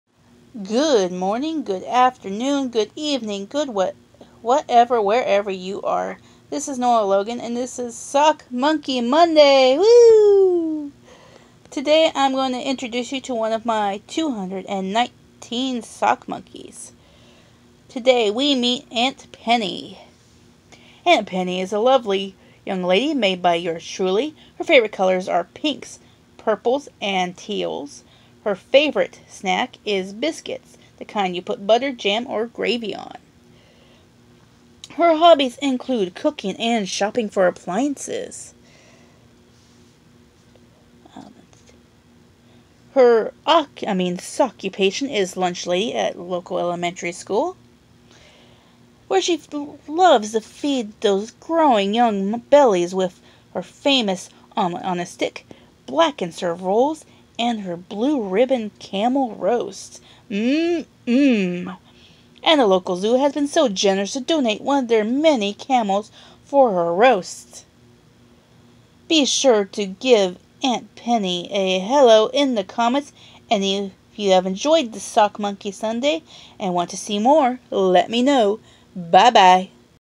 Sock monkey Monday
Almost every Sunday/ Monday we'll meet a Sock monkey from my ever growing collection.
If you would like to see more of my sock monkey collection and monkeys on adventures check out my Instagram beeperandthesockmonkeybots